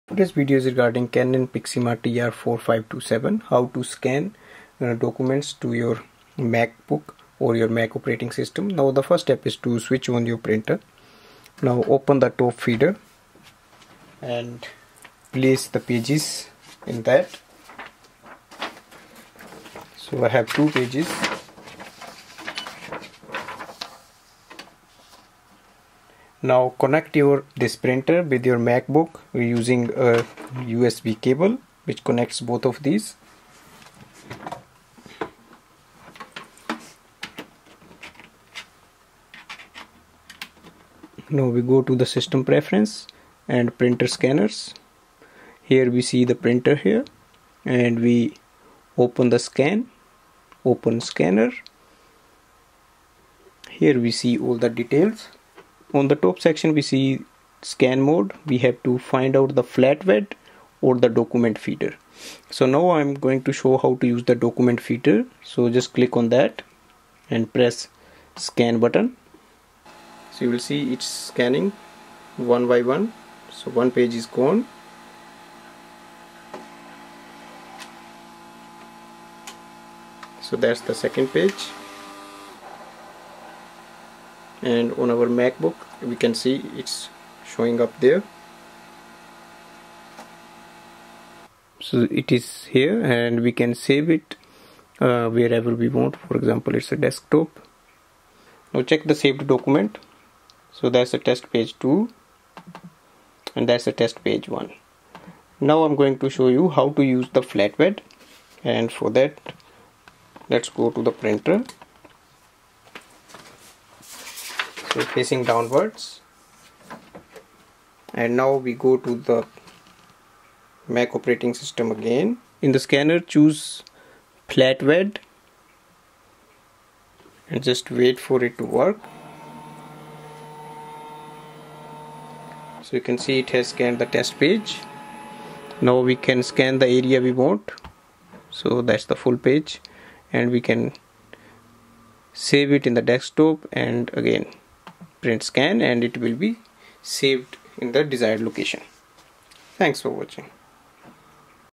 Canon TR4527 Scan To Mac OS review.
This video tutorial reviews easy steps to scan documents to Mac OS using Canon TR4527 Printer.